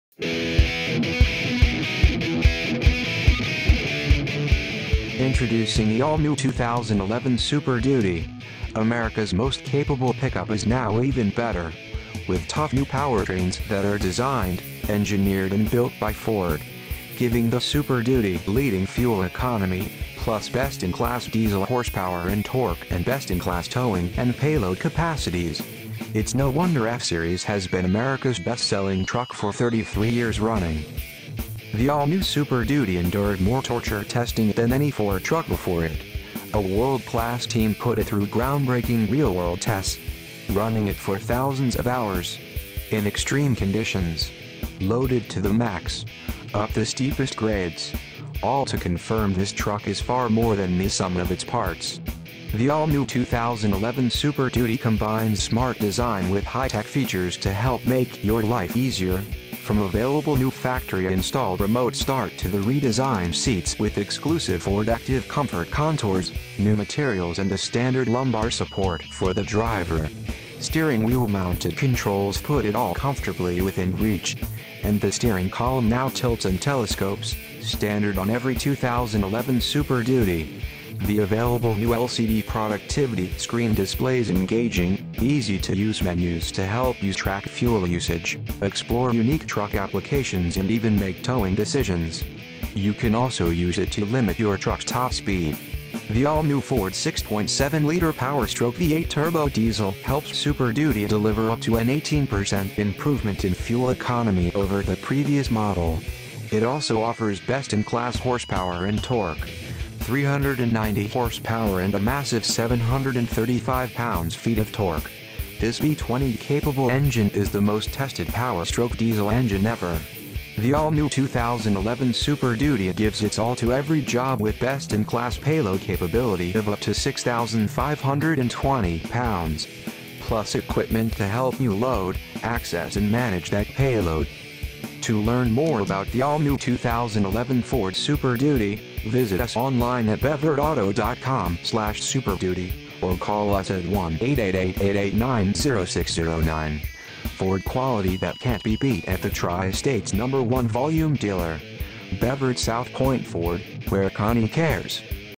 2011 Ford Super Duty Walkaround at Beford South Point Ford
AMERICAS MOST CAPABLE PICKUP is now even better. Tested-tough new powertrains  designed, engineered
and built by Ford  give Super Duty class-leading fuel economy, plus best-in-class diesel horsepower and torque. Best-in-class towing and payload capacities get the job done when no one else can. Standard new Trailer Sway Control helps increase your towing confidence. Its no wonder F-Series has been Americas best-selling truck for 33 years running. And this rugged workhorse is built to make it a whole lot more.